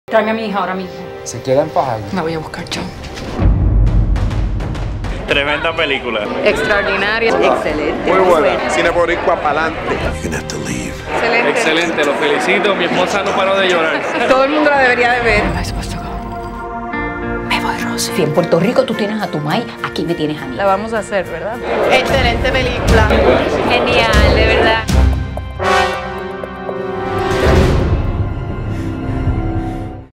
America Premier Ad
We hear people's reaction to the premier of America The Movie.